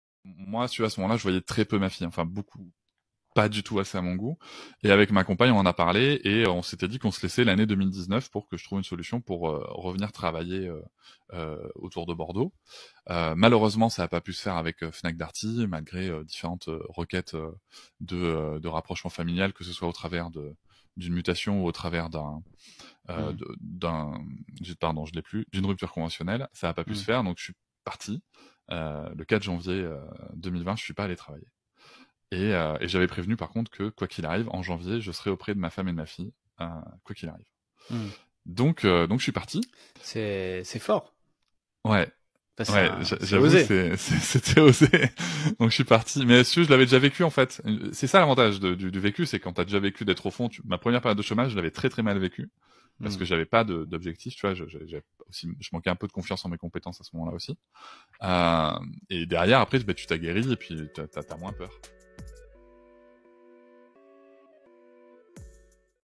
Quitter son poste pour être près des siens, Cédric Rostein a osé et m'explique au micro de followm
Quitter son poste pour être près des siens, Cédric Rostein a osé et m'explique au micro de followmepodcast comment.